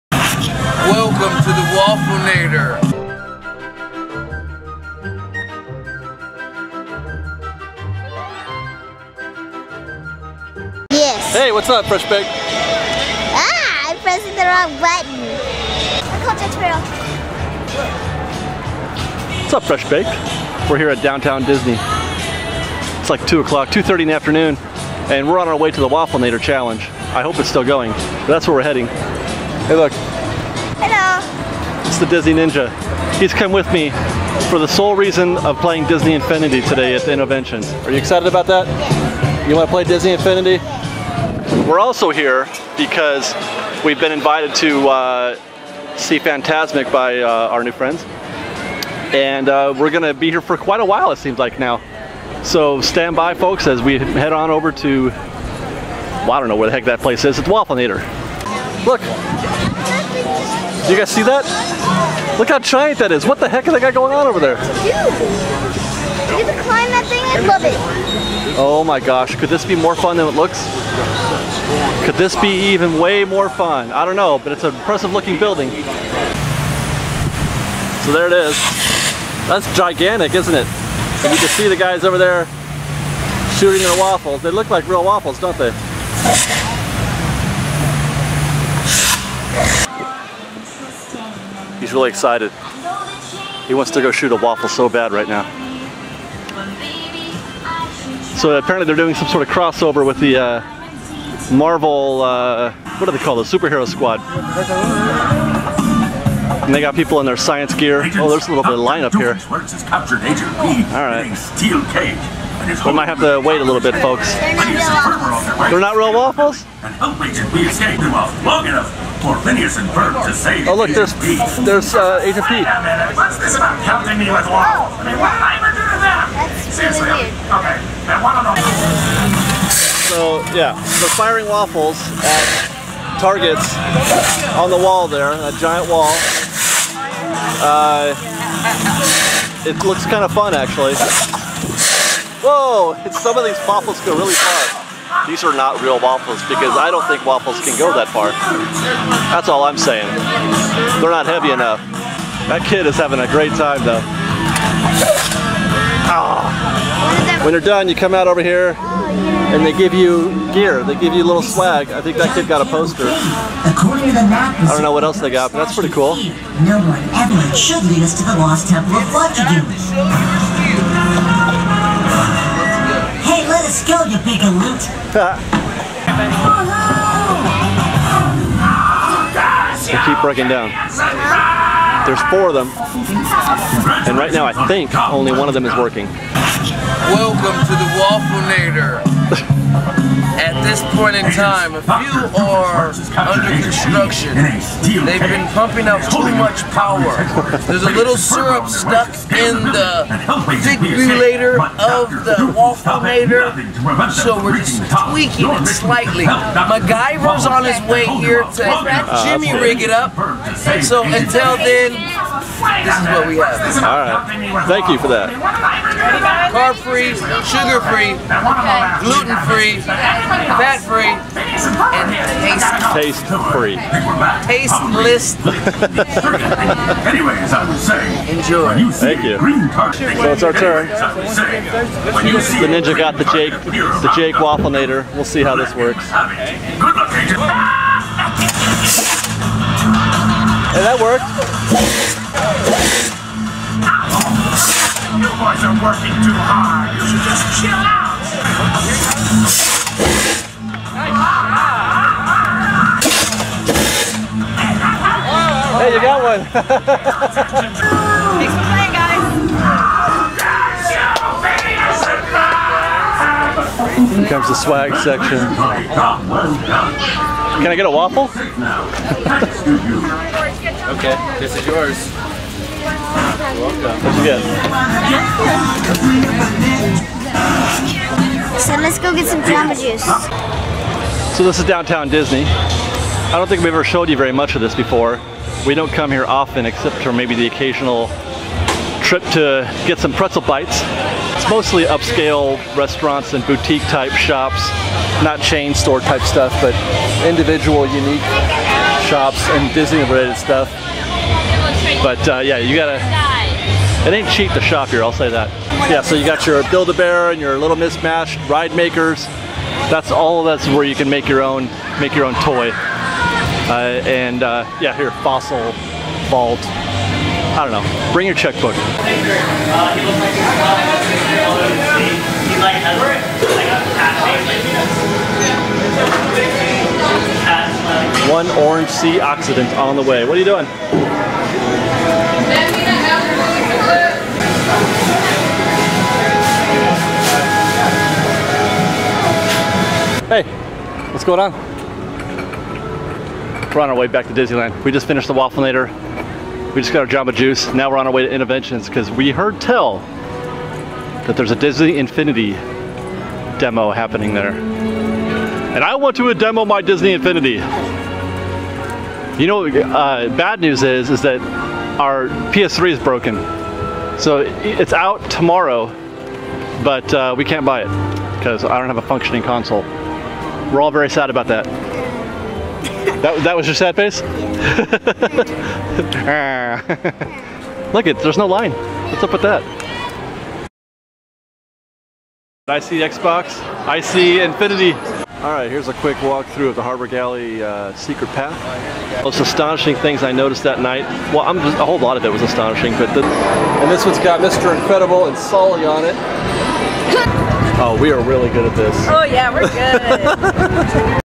Disneyland Update 08.17.13 Pt. 1 | Fresh Baked [HD]
The Disney Ninja and I head to Downtown Disney for the Waffle-inator challenge. This was. pretty cool actually, despite the fact most of the launchers weren't working. All in all, good job Disney!

This video was shot in HD on August 17th, 2013 at Disneyland in Anaheim, California.

for detailed trip reports, ride throughs, special features and more.  Youtube is just the beginning!